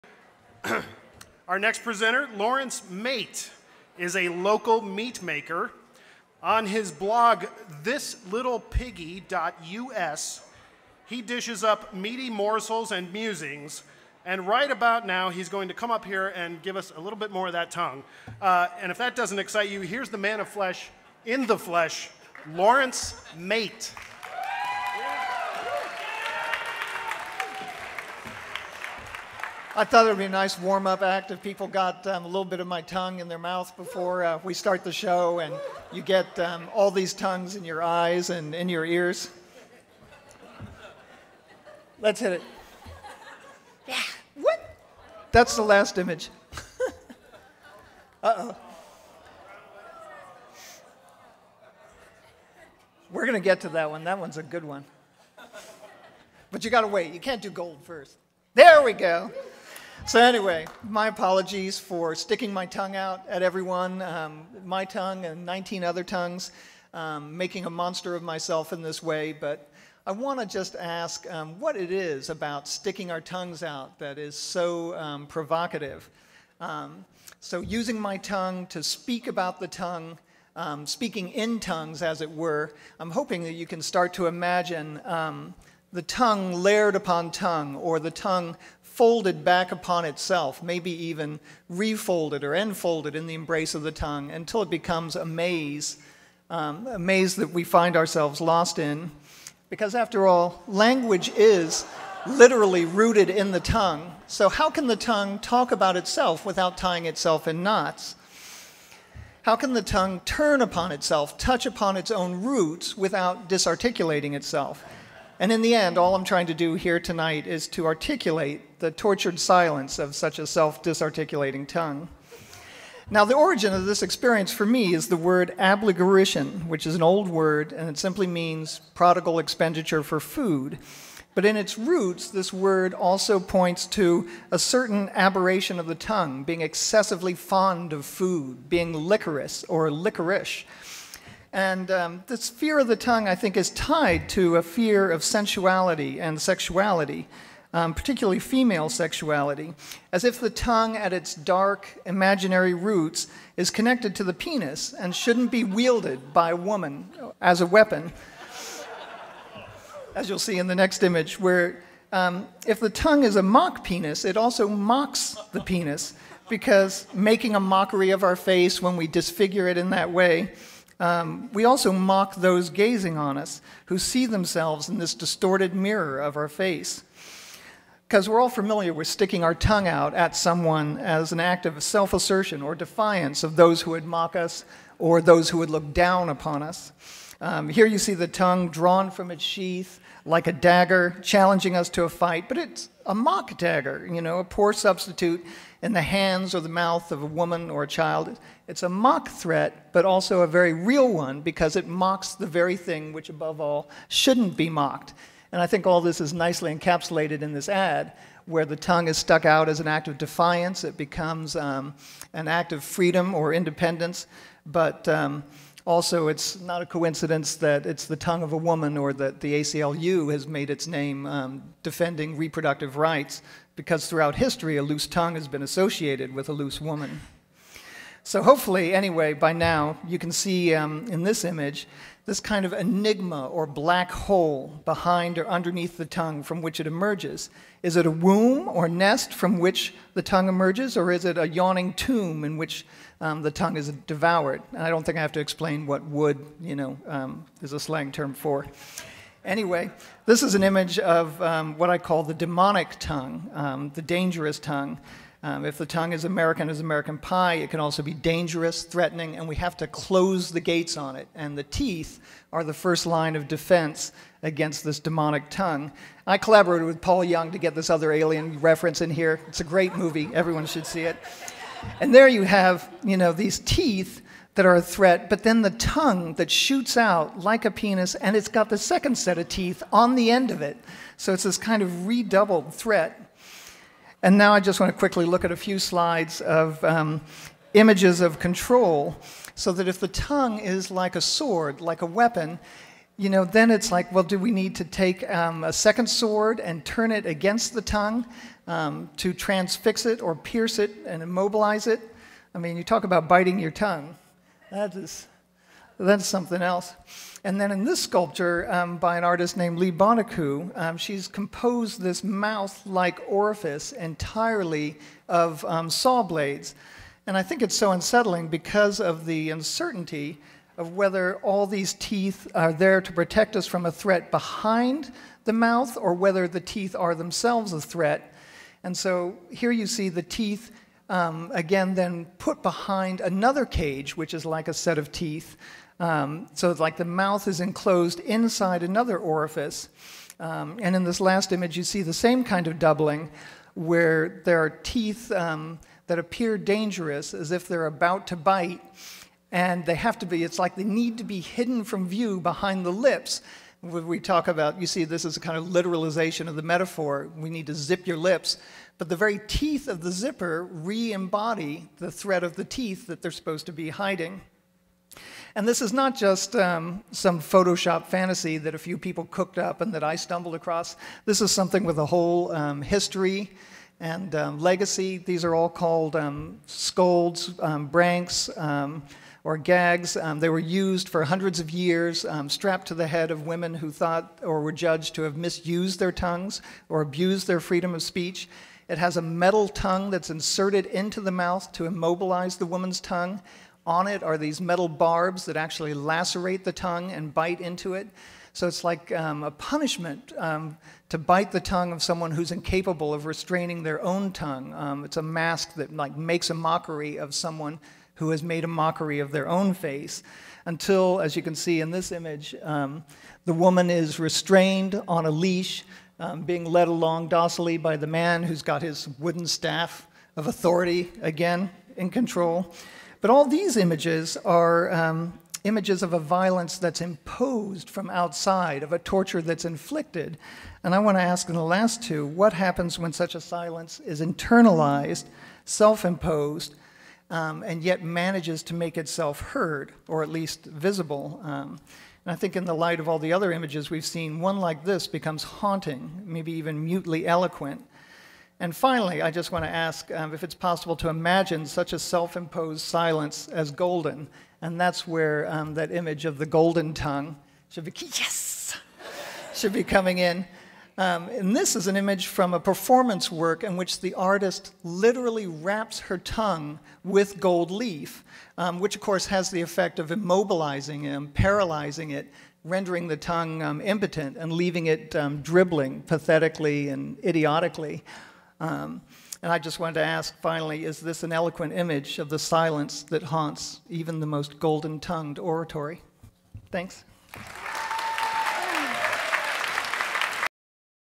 Laurence Mate on abligurition - Pecha Kucha 1/30/2010
Laurence Mate presents on abligurition at PechaKucha vol II at Canopy Club in Urbana IL on 1/30/2010